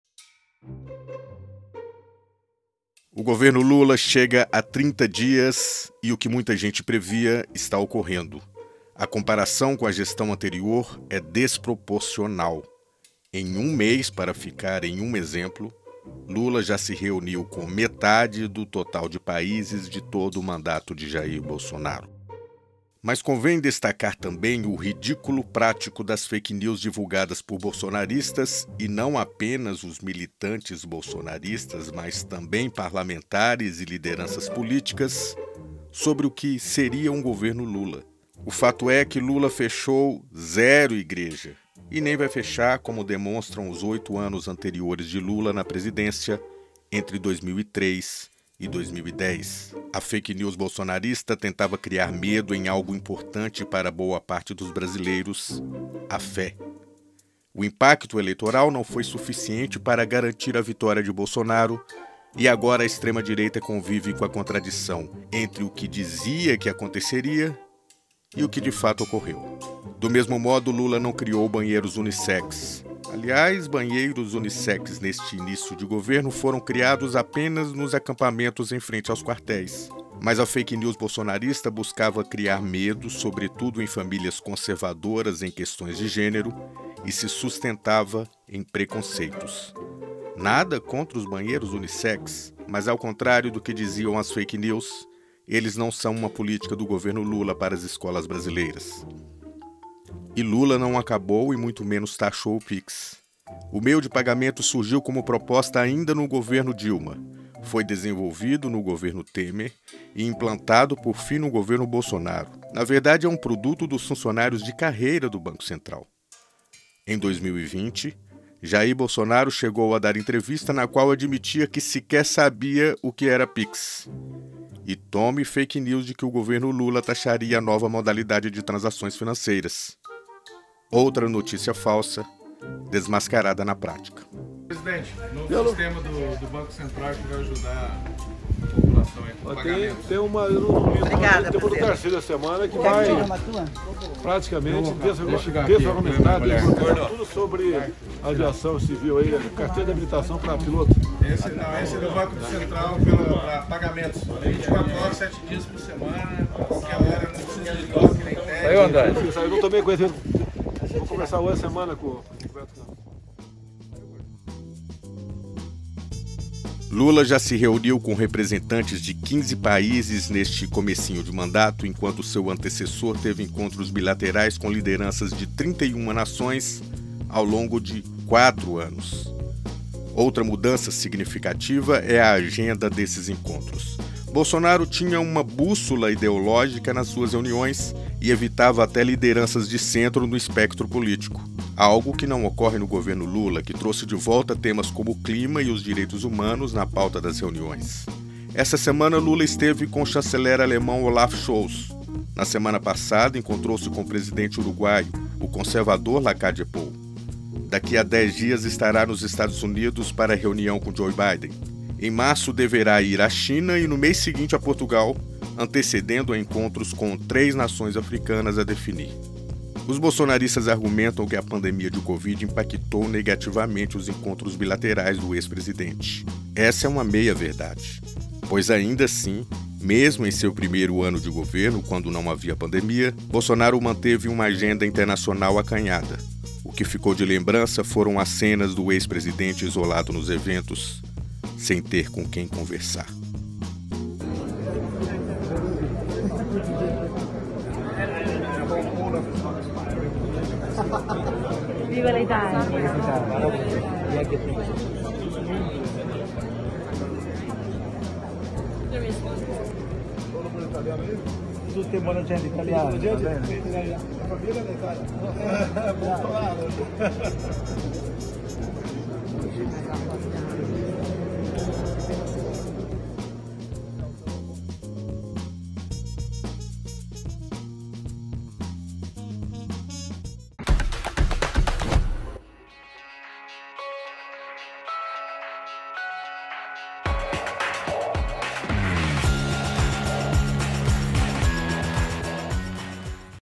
EM UM MÊS, LULA SE REUNIU COM METADE DO TOTAL DE PAÍSES DE TODO O MANDATO DE BOLSONARO | Cortes 247
O governo Lula chega a 30 dias e o que muita gente previa está ocorrendo. A comparação com a gestão anterior é desproporcional. Em um mês, Lula já se reuniu com metade do total de países de todo o mandato de Jair Bolsonaro. Mas convém destacar também o ridículo prático das fake news divulgadas por bolsonaristas sobre o que seria um governo Lula. Confira.